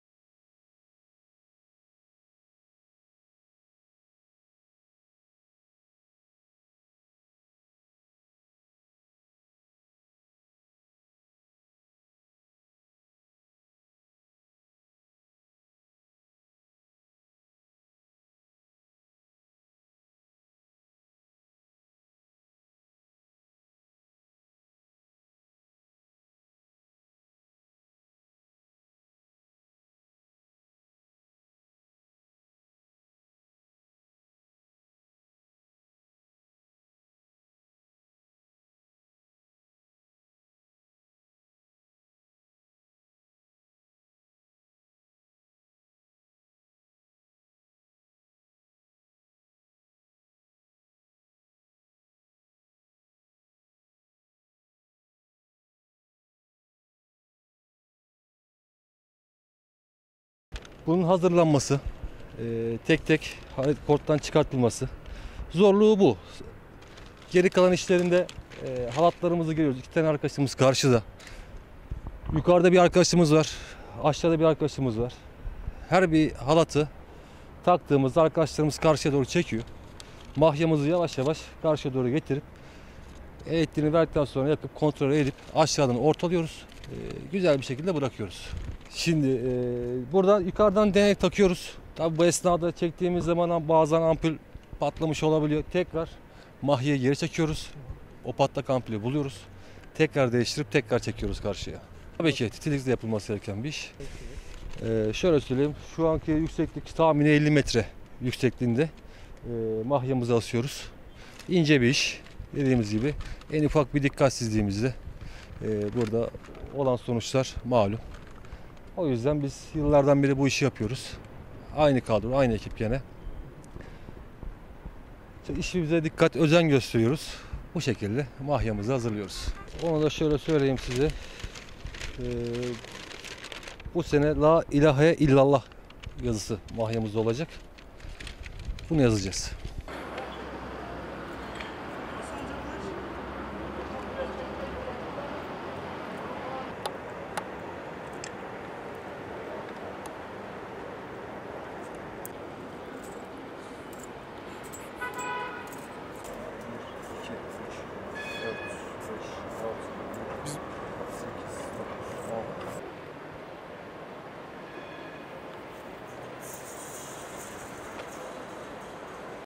Kelime-i Tevhid'i 50 Metre Yükseğe Yazdılar
Eskişehir’deki Reşadiye Camii'nde bu yılkı geleneksel Ramazan ayı öncesi mahya için hazırlıklar tamamlanırken, iki minare arasında 165 adet ampul ile Kelime-i Tevhid, yani ‘La ilahe illallah’ yazılacak.